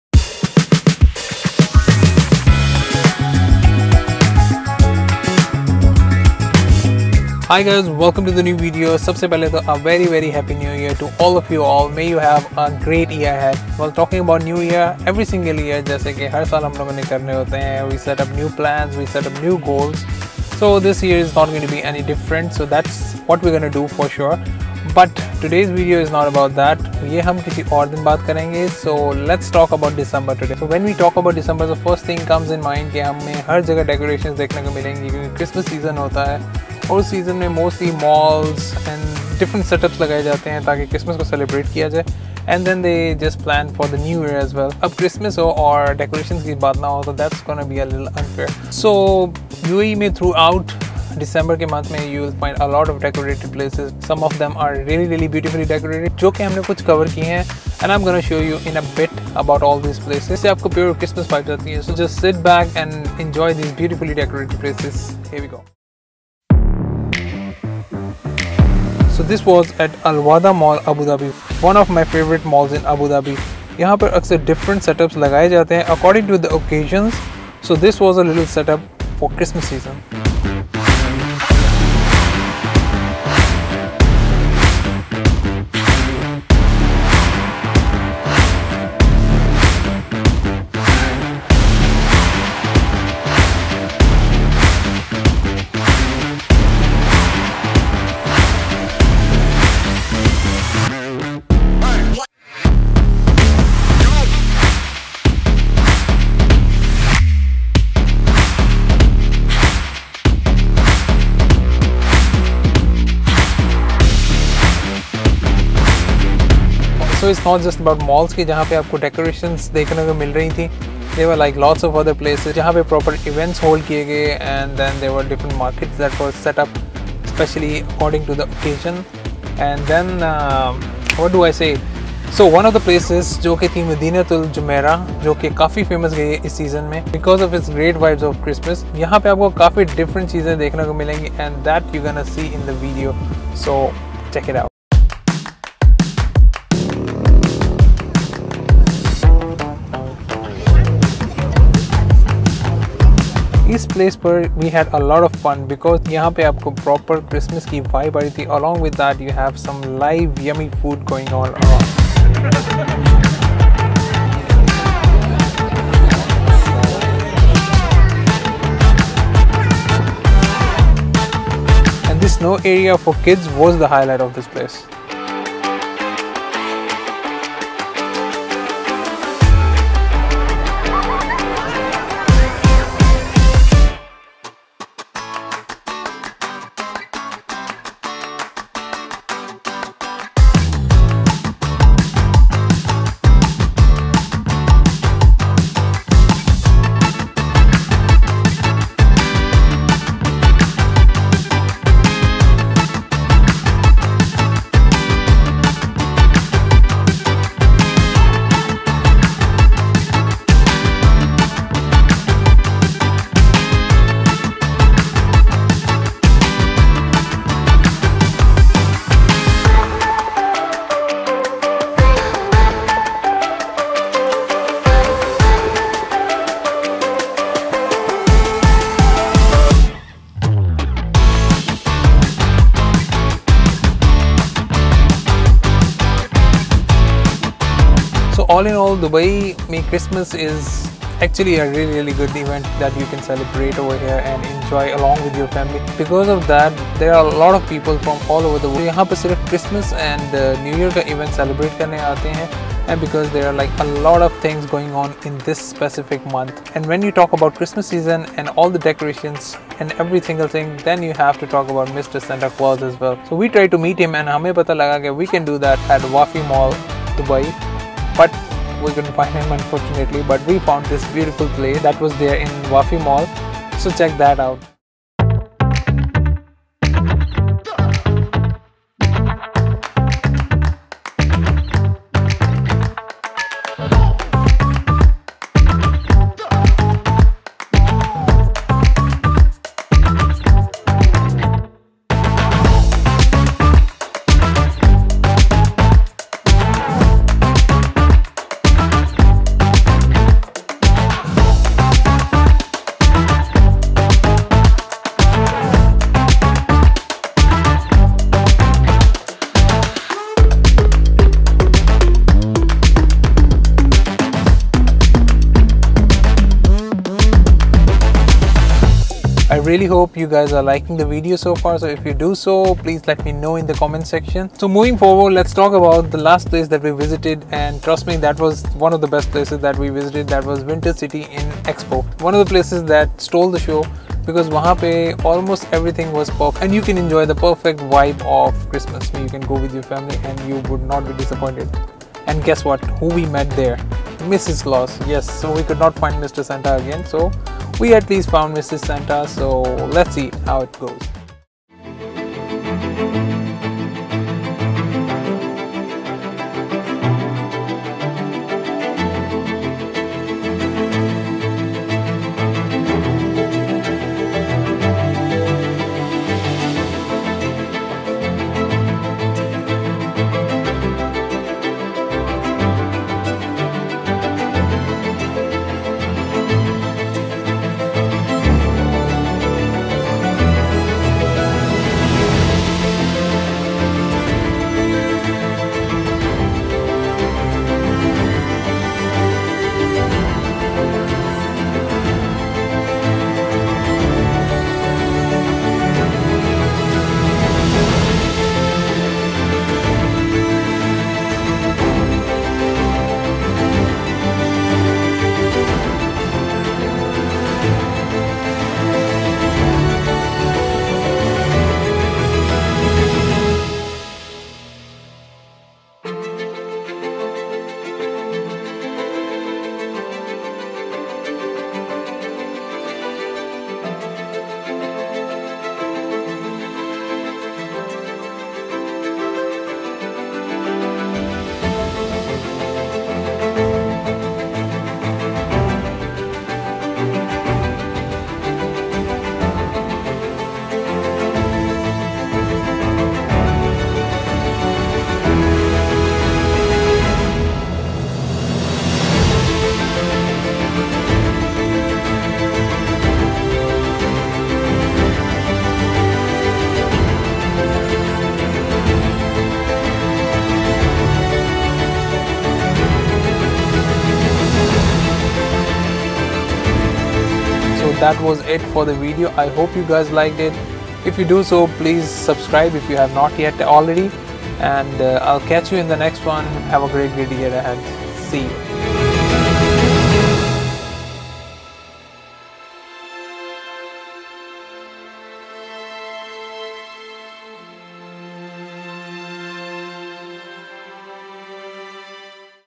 Winters in Dubai - Christmas Season Special
Dubai and its tourism is always on the cards for everyone. So why not let's check some of the activities and the vibe this city offers in December.
As we step in to the New Year let me wish you all a very Happy New Year.
May you all have a great year ahead.